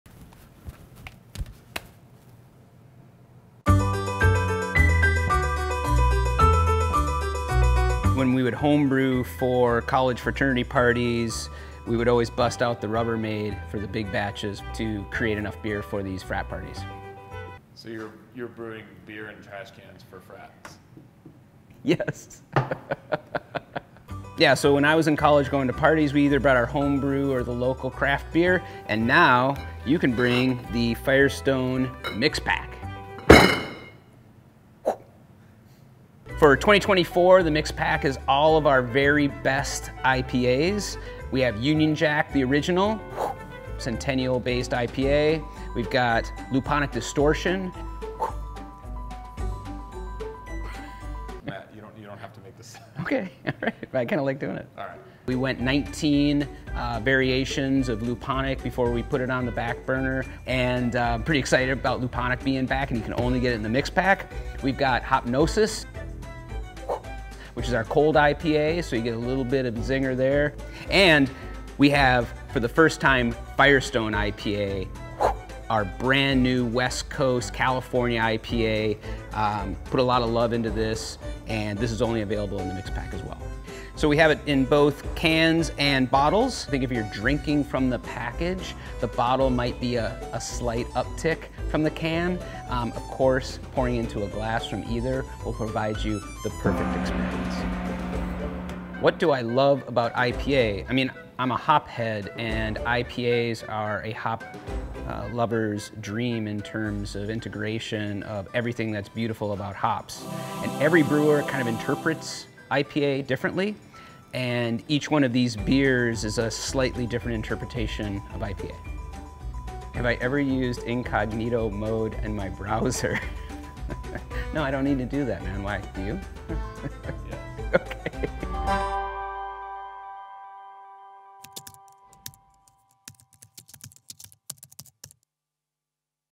Beer Talk with Matt Brynildson: IPA Mixed Pack
Like the mixed plastic bags full of beer you’d bring to a college party, only much, much, much more elevated.

It's time for watered-down, light, tasteless beers to stay on the shelves, because our latest IPA Mixed Pack is here. And it's filled with great beer for those with even better taste. It’s the ultimate crowd-pleaser, and it’s also what we’re showing off in our latest episode of Beer Talk with Brewmaster Matt Brynildson.

“I’m a hop head and IPAs are a hop lover's dream in terms of integration of everything that is beautiful about hops.” — Matt Brynildson

Playing alongside Firestone Walker IPA classics Union Jack and Hopnosis are two fresh features: the revamped return of Luponic Distortion and the all-new Firestone California IPA. Altogether, our IPA Mixed Pack is literally loaded with some seriously flavor-packed beers.

Firestone Walker Brewing Company is proudly brewed in Paso Robles, California. Must be over 21 to drink.